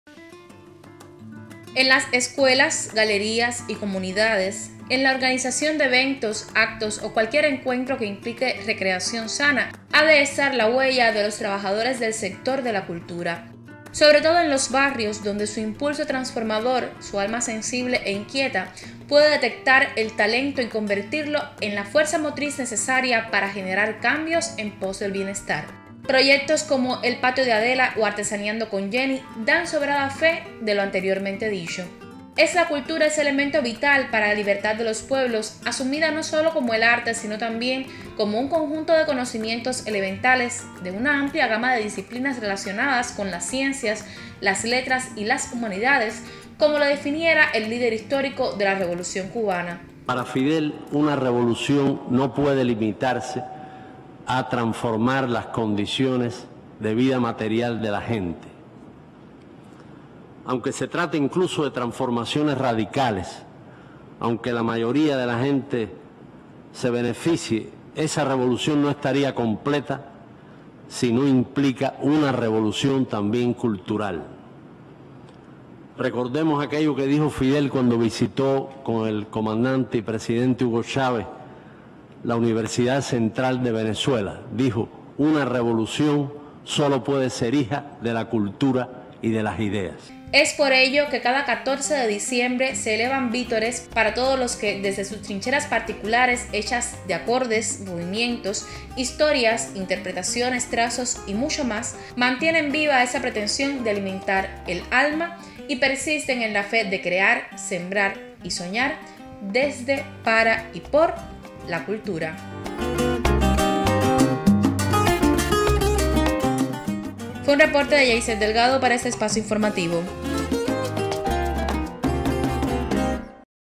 14 de diciembre: Día del Trabajador de la Cultura
Cada 14 de diciembre, Cuba celebra el Día del Trabajador de la Cultura, en saludo al Natalicio de Raúl Gómez García, joven poeta, asaltante al Cuartel Mondaca… A la fecha nos acerca el siguiente reporte